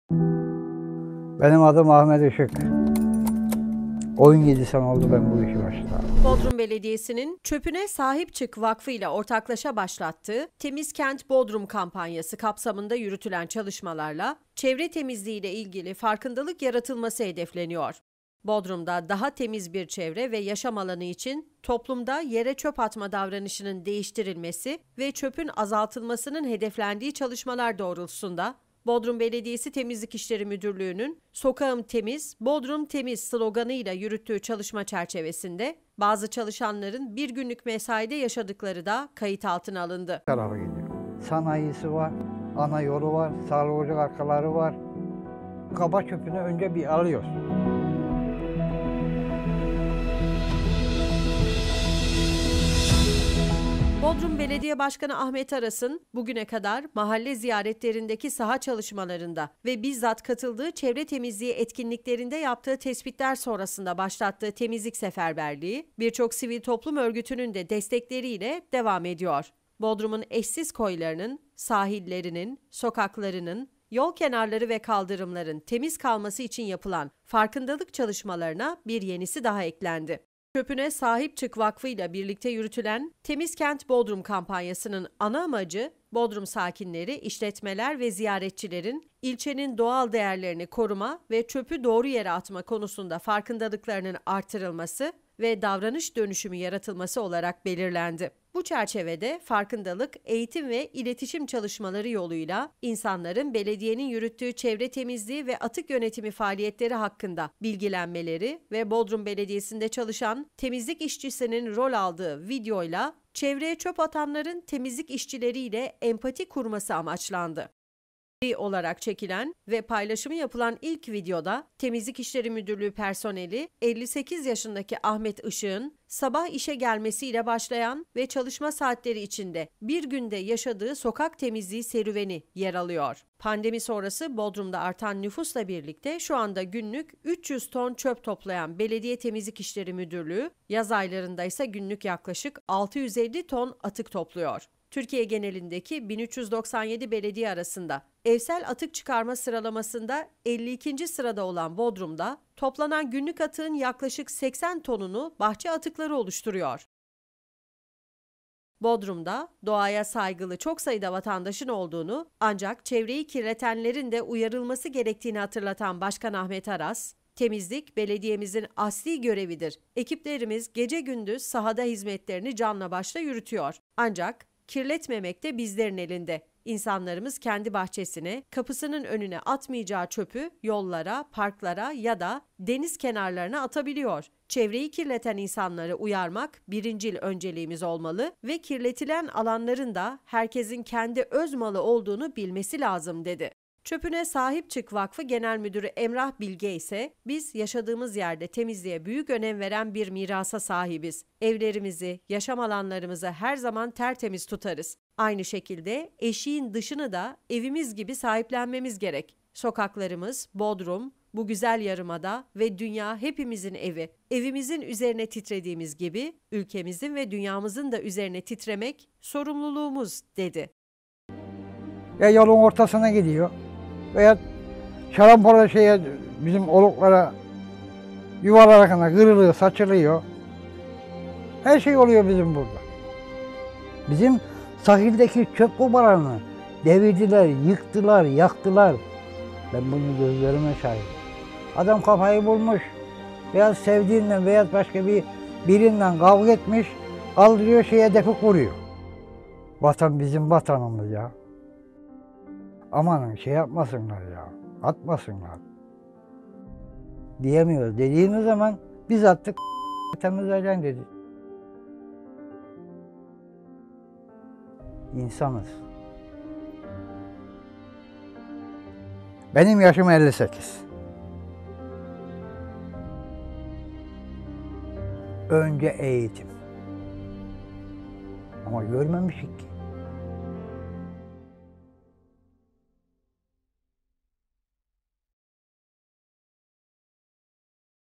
BODRUM BELEDİYESİ'NDEN ÇÖPÜNE SAHİP ÇIK VAKFI İLE ORTAK PROJE
Bodrum Belediyesi'nin, Çöpüne Sahip Çık Vakfı ile ortaklaşa başlattığı, "Temiz Kent Bodrum Kampanyası" kapsamında yürütülen çalışmalarla, çevre temizliği ile ilgili farkındalık yaratılması hedefleniyor.

Bodrum'da, daha temiz bir çevre ve yaşam alanı için toplumda yere çöp atma davranışının değiştirilmesi ve çöpün azaltılmasının hedeflendiği çalışmalar doğrultusunda, Bodrum Belediyesi Temizlik İşleri Müdürlüğü'nün "Sokağım Temiz, Bodrum Temiz" sloganı ile yürüttüğü çalışma çerçevesinde, bazı çalışanların bir günlük mesaide yaşadıkları da kayıt altına alındı.

Bodrum Belediye Başkanı Ahmet Aras'ın, bugüne kadar mahalle ziyaretlerindeki saha çalışmalarında ve bizzat katıldığı çevre temizliği etkinliklerinde yaptığı tespitler sonrasında başlattığı temizlik seferberliği, birçok sivil toplum örgütünün de destekleriyle devam ediyor. Bodrum'un eşsiz koylarının, sahillerinin, sokaklarının, yol kenarları ve kaldırımların temiz kalması için yapılan farkındalık çalışmalarına bir yenisi daha eklendi.

Çöpüne Sahip Çık Vakfı ile birlikte yürütülen "Temiz Kent Bodrum Kampanyası" nın ana amacı Bodrum sakinleri, işletmeler ve ziyaretçilerin ilçenin doğal değerlerini koruma ve çöpü doğru yere atma konusunda farkındalıklarının artırılması ve davranış dönüşümü yaratılması olarak belirlendi. Bu çerçevede farkındalık, eğitim ve iletişim çalışmaları yoluyla, insanların Belediyenin yürüttüğü çevre temizliği ve atık yönetimi faaliyetleri hakkında bilgilenmeleri ve Bodrum Belediyesi’nde çalışan temizlik işçisinin rol aldığı videoyla çevreye çöp atanların temizlik işçileri ile empati kurması amaçlandı.

Seri olarak çekilen ve paylaşımı yapılan ilk videoda, Temizlik İşleri Müdürlüğü personeli 58 yaşındaki Ahmet Işık'ın sabah işe gelmesiyle başlayan ve çalışma saatleri içinde bir günde yaşadığı sokak temizliği serüveni yer alıyor.

Pandemi sonrası Bodrum'da artan nüfus ile birlikte şu anda günlük 300 ton çöp toplayan Belediye Temizlik İşleri Müdürlüğü yaz aylarında ise günlük yaklaşık 650 ton atık topluyor. Türkiye genelindeki bin 397 belediye arasında evsel atık çıkarma sıralamasında 52. sırada olan Bodrum'da toplanan günlük atığın yaklaşık 80 tonunu bahçe atıkları oluşturuyor.

Bodrum'da doğaya saygılı çok sayıda vatandaşın olduğunu ancak çevreyi kirletenlerin de uyarılması gerektiğini hatırlatan Başkan Ahmet Aras, "Temizlik, Belediyemizin asli görevidir. Ekiplerimiz, gece gündüz sahada hizmetlerini canla başla yürütüyor. Ancak kirletmemek de bizlerin elinde. İnsanlarımız kendi bahçesine, kapısının önüne atmayacağı çöpü yollara, parklara ya da deniz kenarlarına atabiliyor. Çevreyi kirleten insanları uyarmak birincil önceliğimiz olmalı ve kirletilen alanların da herkesin kendi öz malı olduğunu bilmesi lazım." dedi.

Çöpüne Sahip Çık Vakfı Genel Müdürü Emrah Bilge ise “Biz yaşadığımız yerde temizliğe büyük önem veren bir mirasa sahibiz, evlerimizi, yaşam alanlarımızı her zaman tertemiz tutarız. Aynı şekilde eşiğin dışını da evimiz gibi sahiplenmemiz gerek. Sokaklarımız,Bodrum, bu güzel yarımada ve dünya hepimizin evi. Evimizin üzerine titrediğimiz gibi ülkemizin ve dünyamızın da üzerine titremek sorumluluğumuz.”dedi.